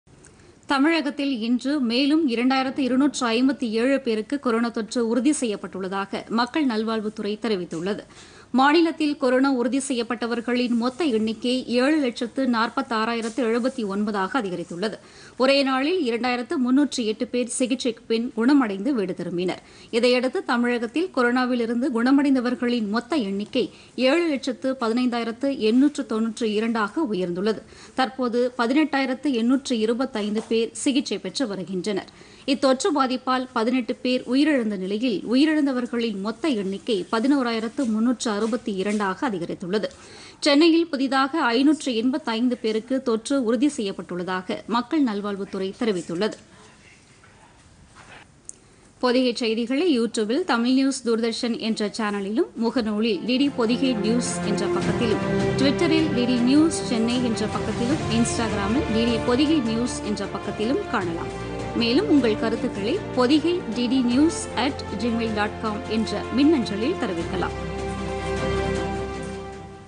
ஒரே நாளில் 2,257 பேருக்கு தொற்று
தமிழகத்தில் இன்று ஒரே நாளில் இரண்டாயிரத்து 257 பேருக்கு கொரோனா தொற்று -  ஏழு லட்சத்து  15 ஆயிரத்து 892 பேர் குணமடைந்தனர் :  சென்னையில் 575 பேருக்கு பாதிப்பு